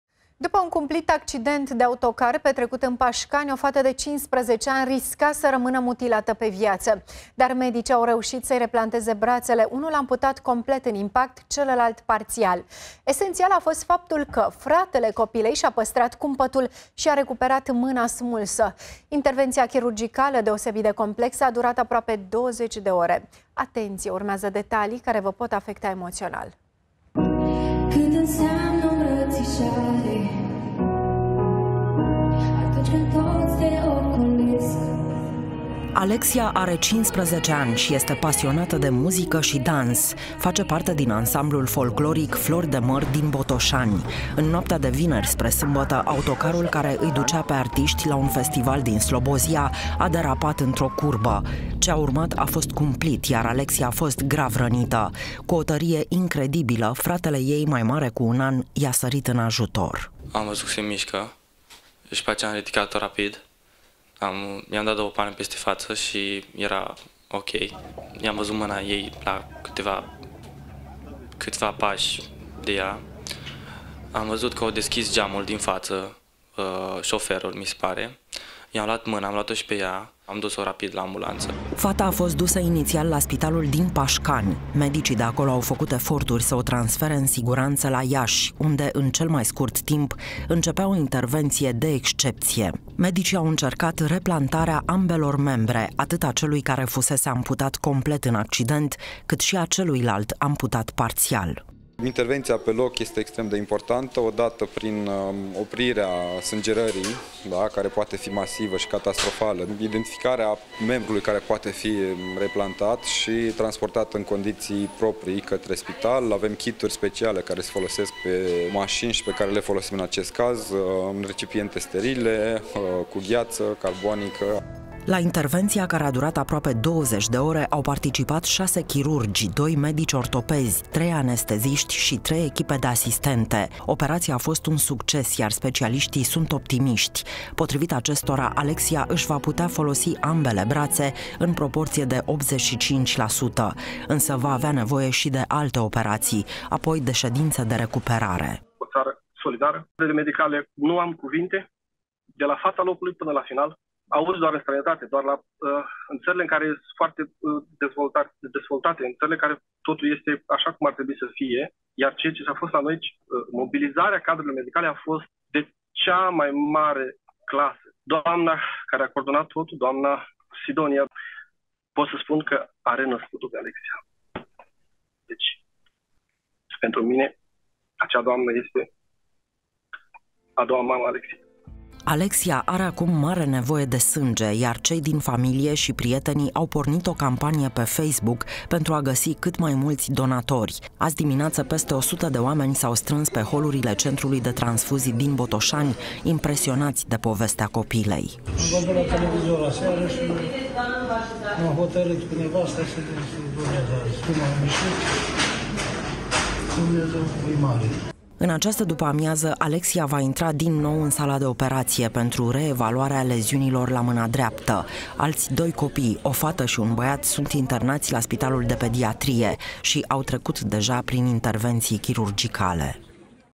Alexia, fata de 15 ani cu brațele amputate, salvată datorită fratelui care i-a recuperat mâna smulsă
După un cumplit accident de autocar petrecut în Paşcani, o fată de 15 ani risca să rămână mutilată pe viață.